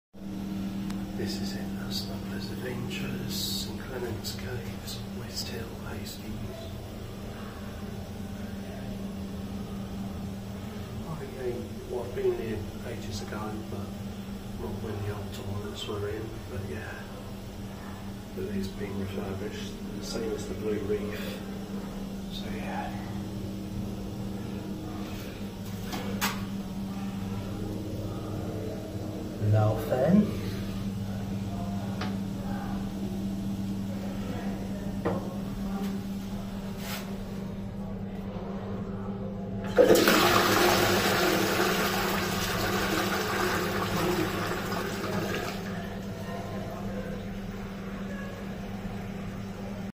Toilets in the Smugglers Adventures St Clements caves West Hill Hastings
This is in of Hastings attractions and smugglers Adventures and Hastings castle and blue reef aquarium are all joint attractions the toilets have been refurbished recently and they used to be 2 Fordham Sprite cisterns one having a Royal Daulton Ashdown pan and the other having a Shires Denbigh pan but don't know what the ladies had back then but now they are done up and I believe that the male and female equivalents had changed sides and the Gents Equivalent has 1 cubicle now and they are Laufen fixtures just like in the blue reef aquarium and the flush was good